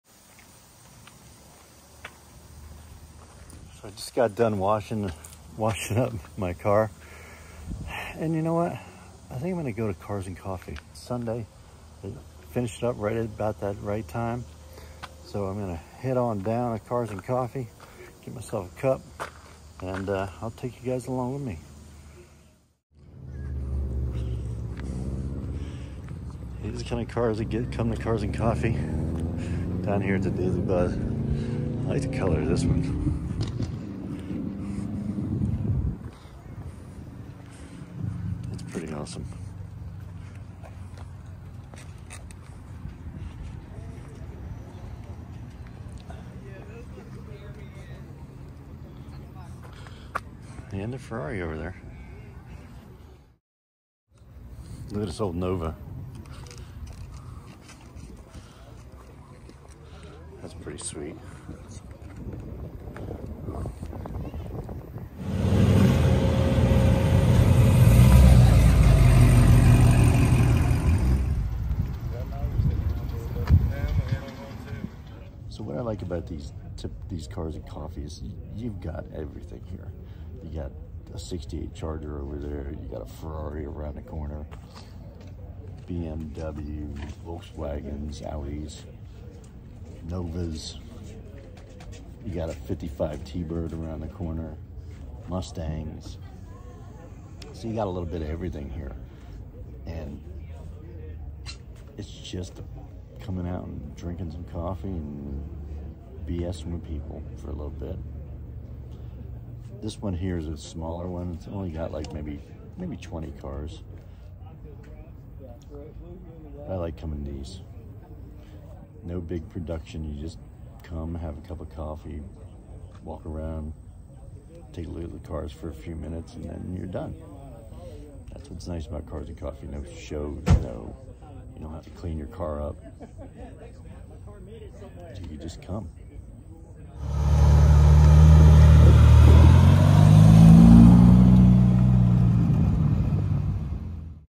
Cars & Coffee - Daily Buzz in Stephen’s City, VA
Admittedly, this is a short video.  But that’s the point of Cars & Coffee. No big production, no entry fee, no awards.  It’s just a get-together to chat about cars over a cup of Joe.  No big whoop.

This time I head to a local spot in Stephen’s City, VA called the Daily Buzz.  The owner is a car guy, and a big promoter of the local car scene.  Really nice guy.  I try to support his business whenever possible.  He holds this Cars & Coffee on Sunday mornings from 10-12.  Sometimes it can get very large, others just a few cars.  But it’s always cool!

So sit back in your easy chair for the next two and a half minutes while this old man shows you around the local car scene.